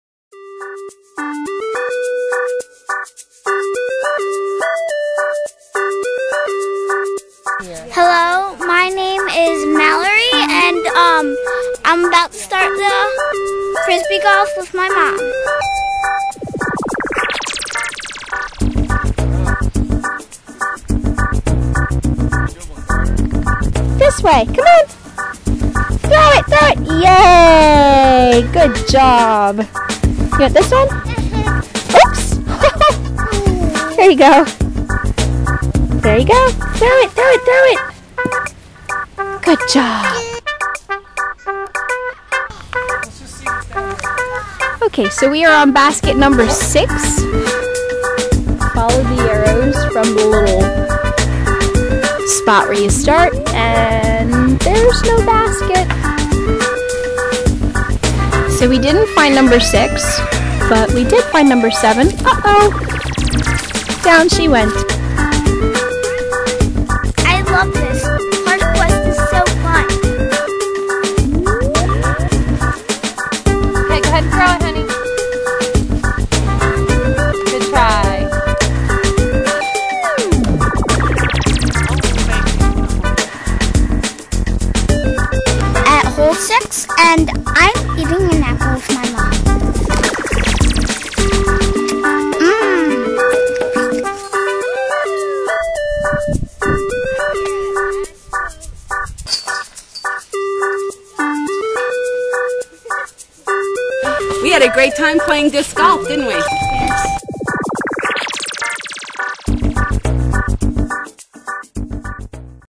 Park Quest: Patapsco Valley State Park
Watch Park Quest teams play disc golf at Patapsco Valley State Park. To learn more about Maryland's state parks and the upcoming Ken Burns documentary "The National Parks: America's Best Idea,"